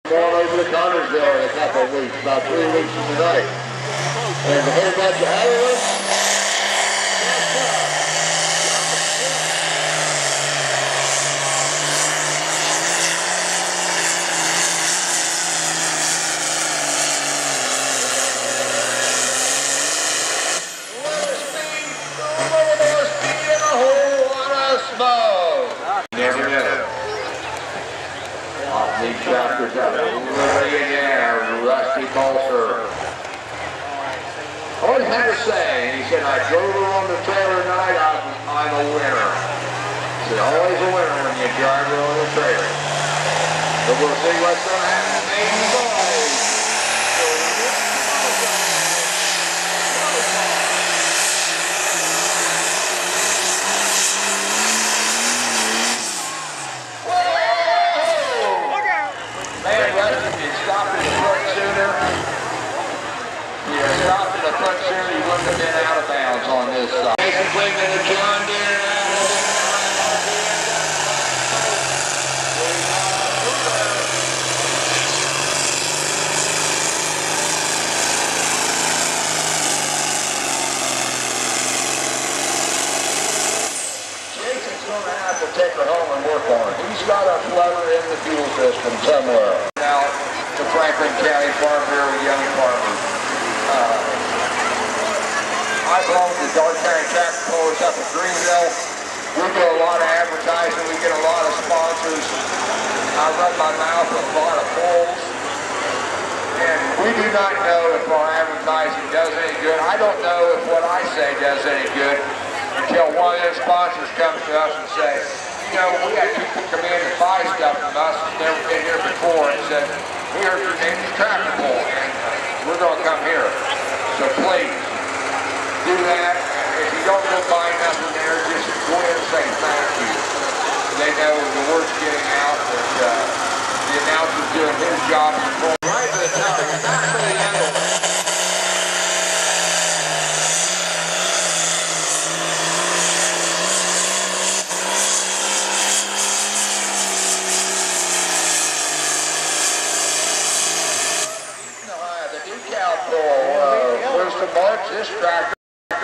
FRANKLIN COUNTY, IN YOUNG FARMERS PRO FARM TRACTOR CLASS
THE ENTIRE FRO FARM TRACTOR CLASS AT THE 2009 EDITION OF THE FRANKLIN COUNTY, IN YOUNG FARMERS TRUCK AND TRACTOR PULL